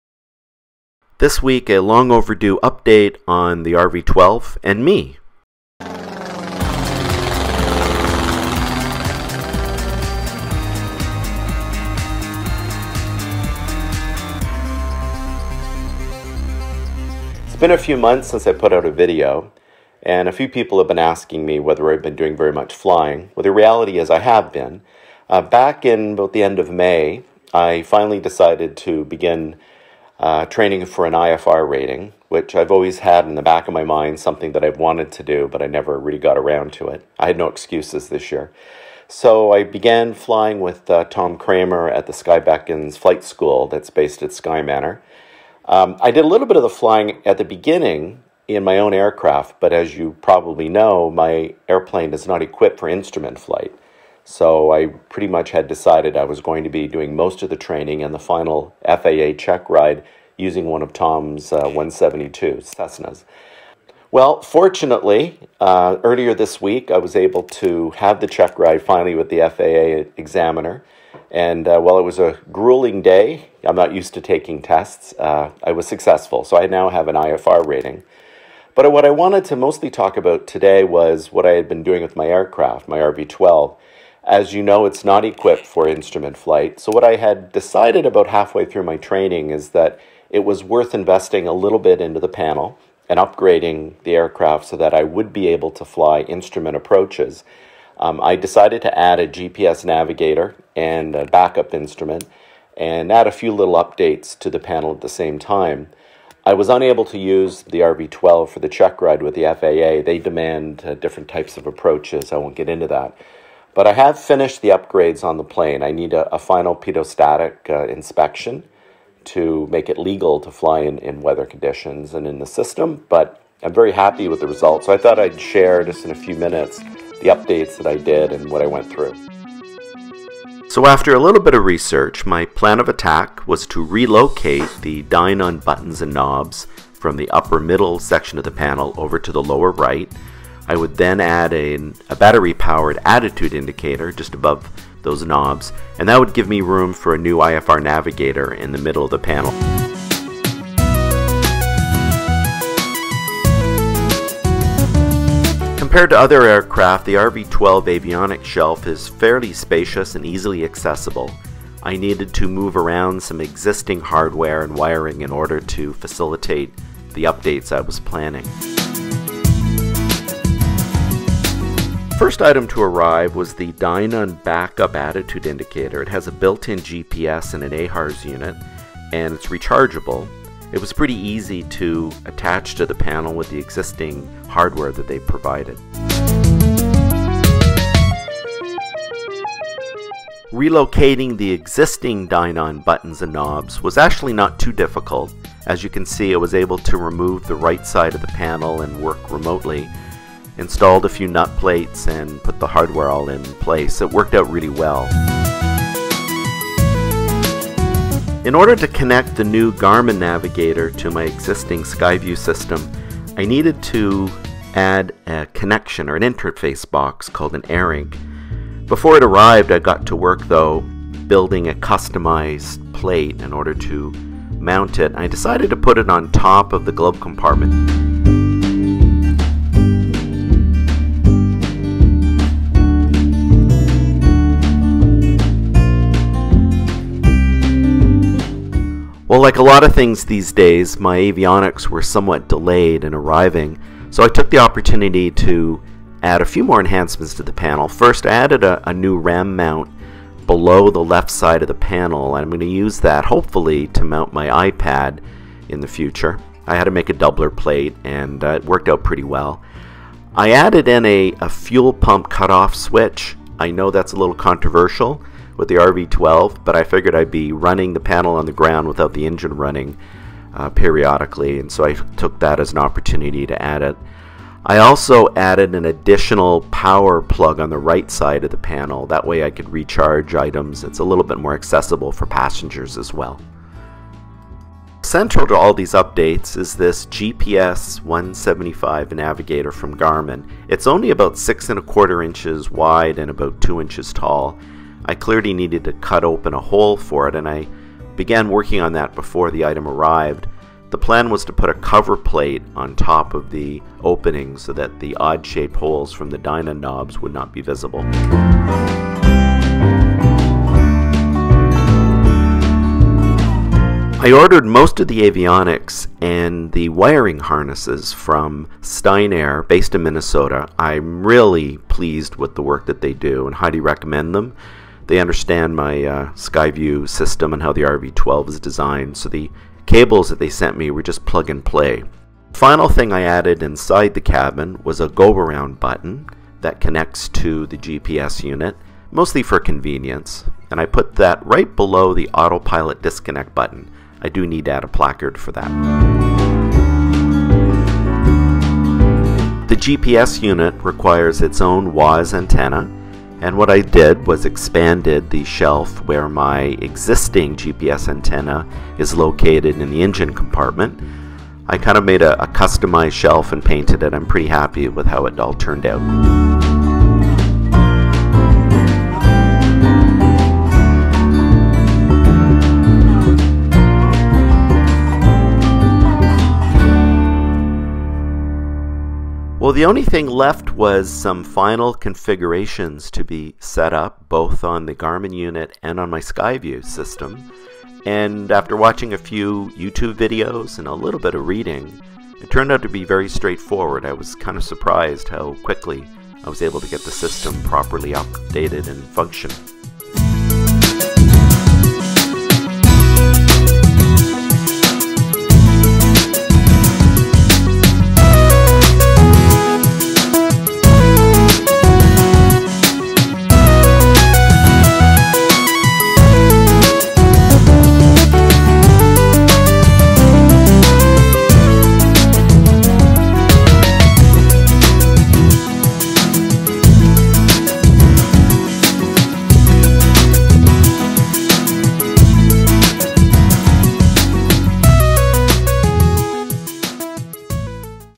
WFA - Upgrade RV-12 Panel with IFR GPS Navigator
Overview of my RV-12 panel upgrade, adding Garmin 175 GPS Navigator, Dynon D3 Attitude Indicator, and misc. other additions.